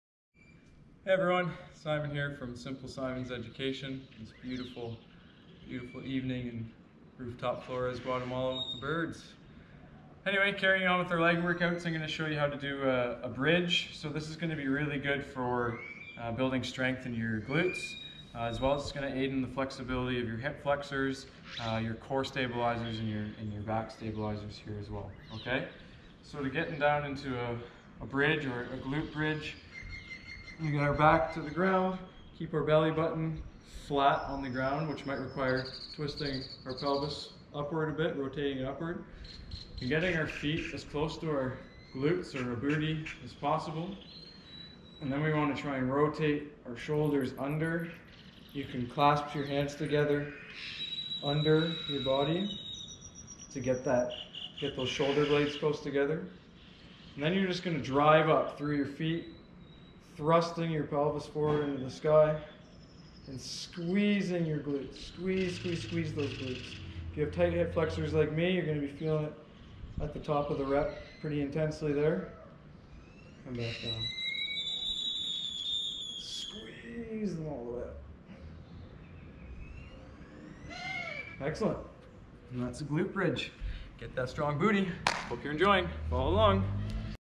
How To Do A Glute Bridge - Leg Body Weight Exercise Tutorials
Simon from Simple Simon's Education teaches you how to do a glute bridge with proper technique.

You can also follow him on Instagram @simonerlich or on Facebook